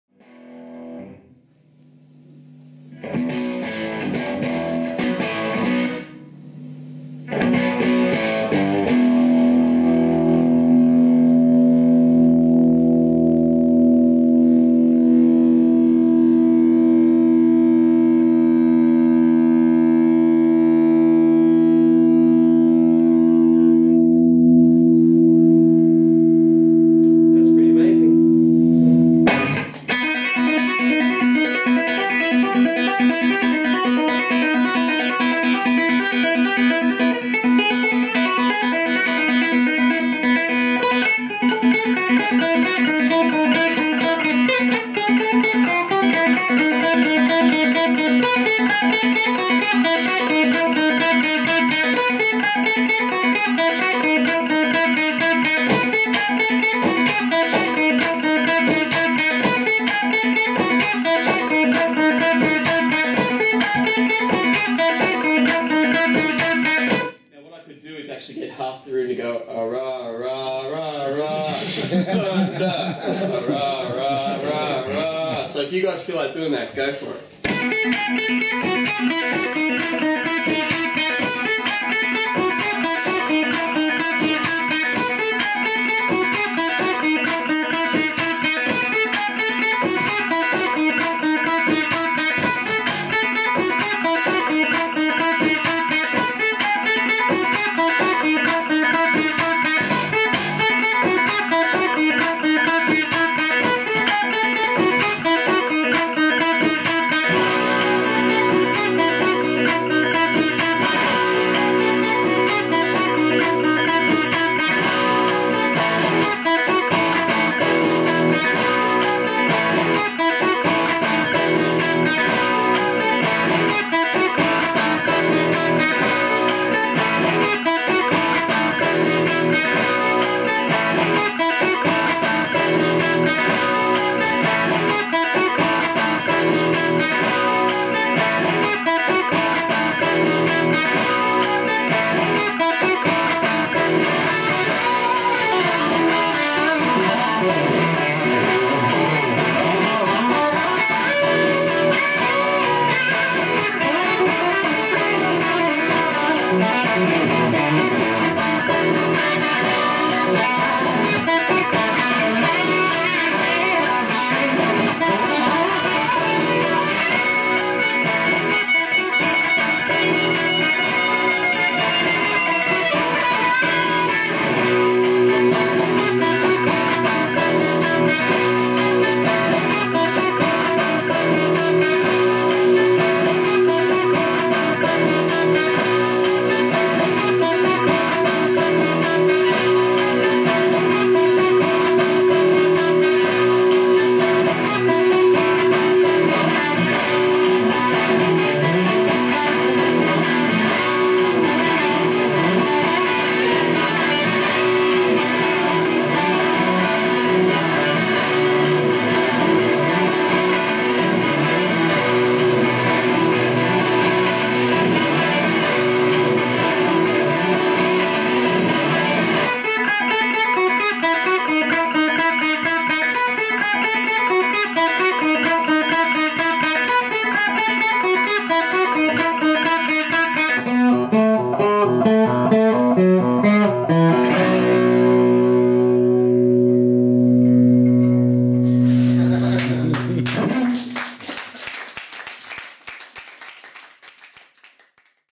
Guitars Addicts Premier Builders Guild Tour 2014
Adam Miller joue sur B3 Fire SL et Koll Troubadour
sur ampli Toneking 45w combo.